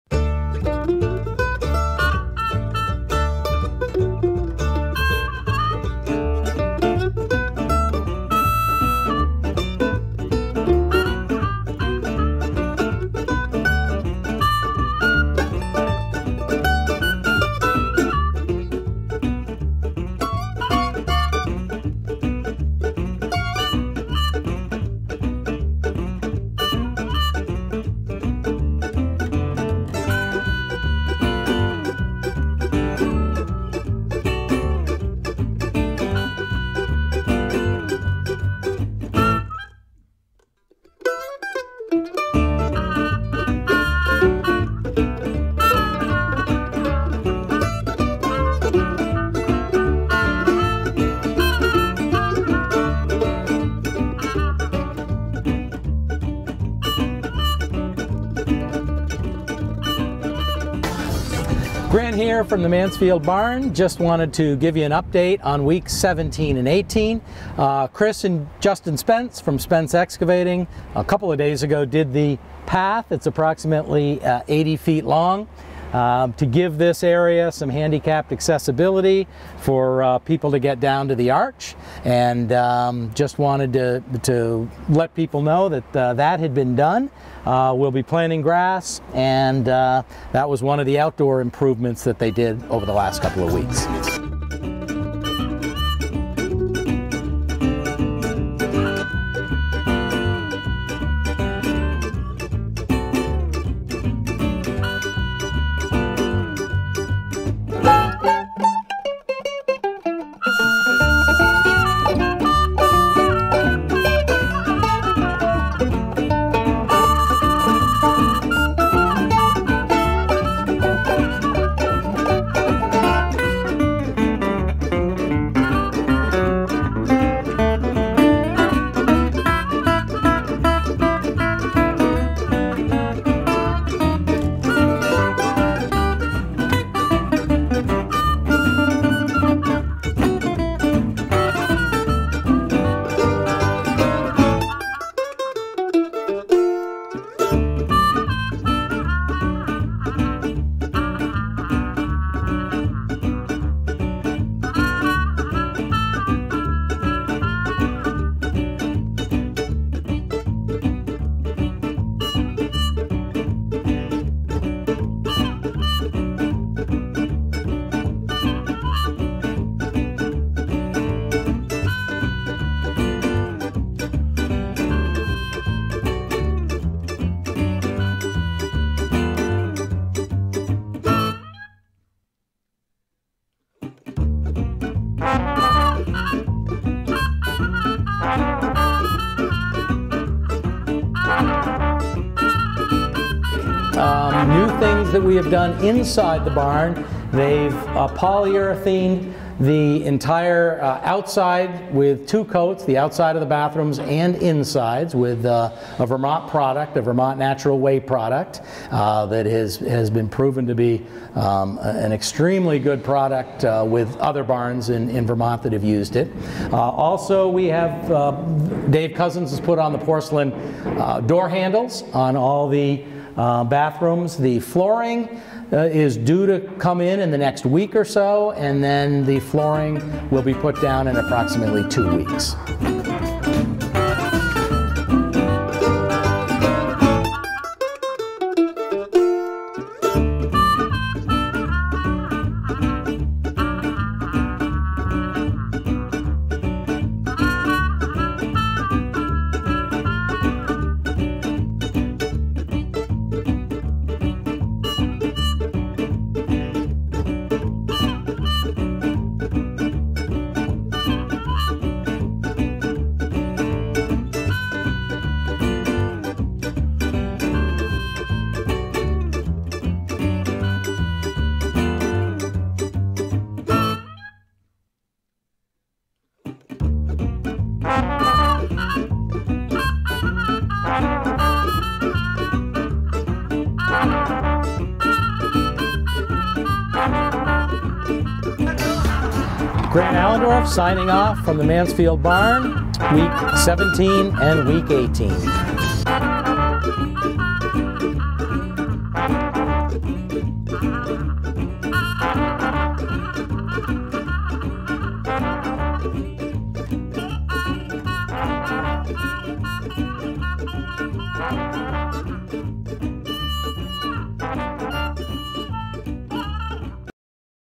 Episode 17 - Ceremony Site
Here we take a peak at the ceremony site, gutter drainage pipes (gutters not installed yet, and the bathrooms are still coming along!!!